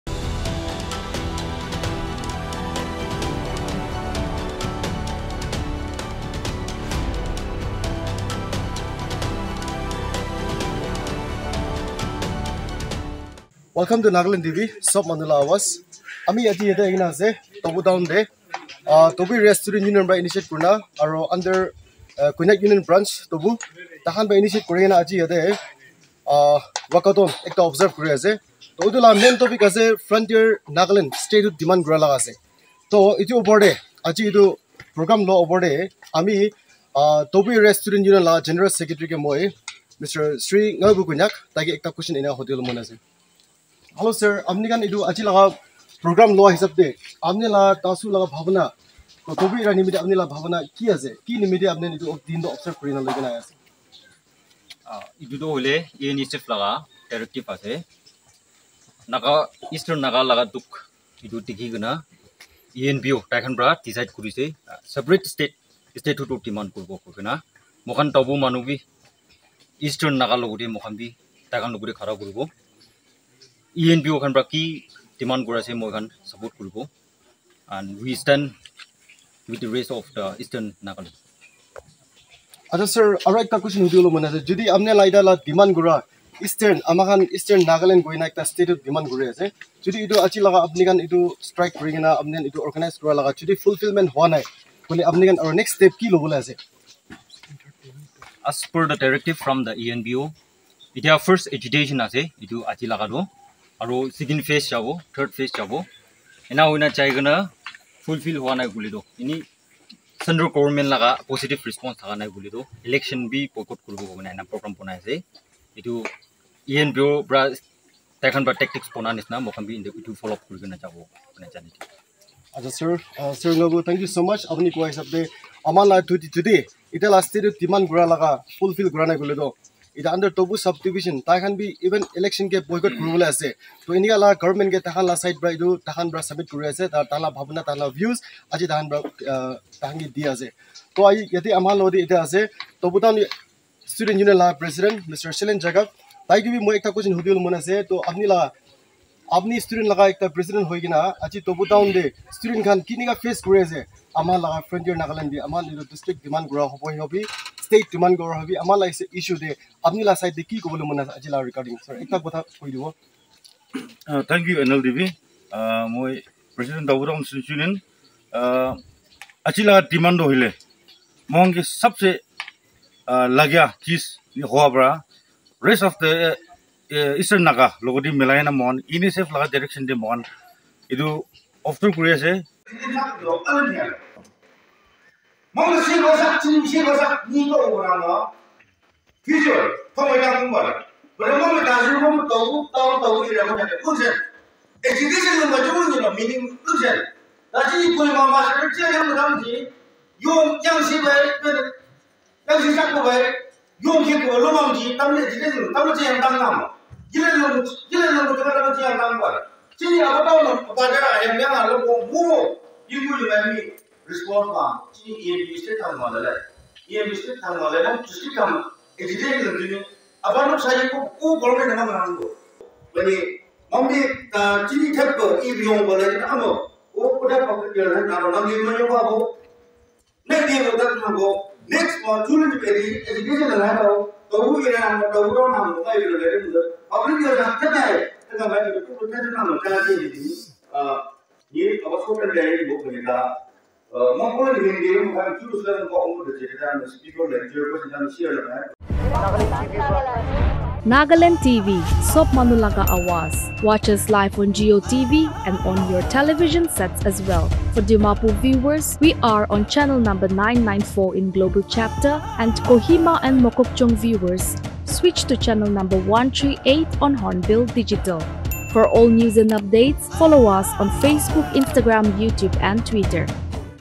TOBU ORGANISES MASS PUBLIC WALKATHON IN DEMAND FOR FRONTIER NAGALAND
A mass public walkathon was organised by Tobu Area Students Union (TASU) and Civil Societies (CSs) at Tobu on October 7. The main aim behind the walkathon was to show support to Eastern Nagaland People's Organisation’s (ENPO) demand for Frontier Nagaland which is a long cherished dream for young educated Nagas of Eastern Nagaland. Tobu NGOs informed that the public would boycott the upcoming election 2023 if their demand was not fulfilled